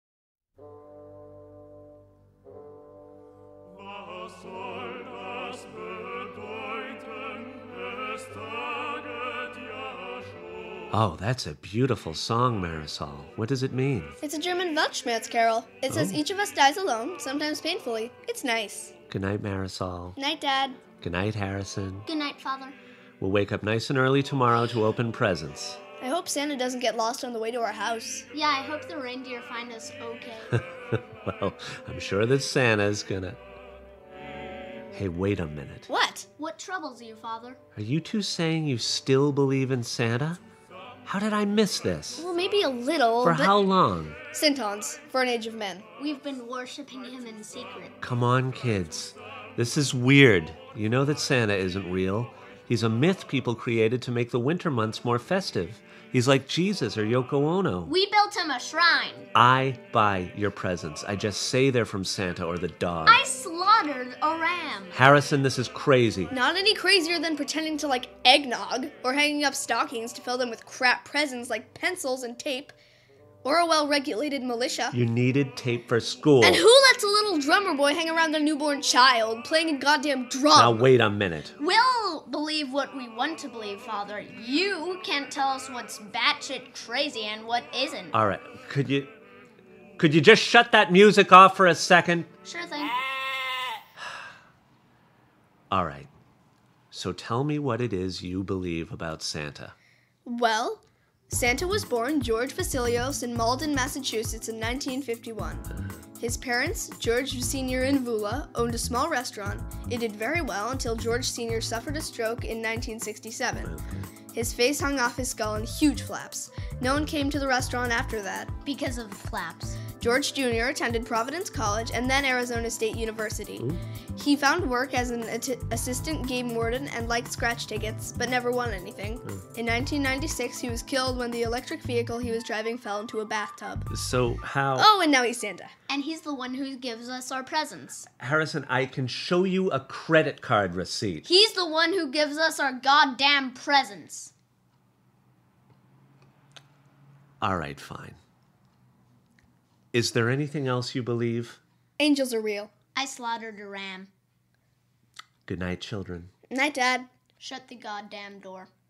I Slaughtered A Ram
A family discusses the origins of Santa and the awesome power of belief. Featuring Marty Barrett, Marisol Barrett, and Harrison Barrett, with artwork by Marisol Barrett.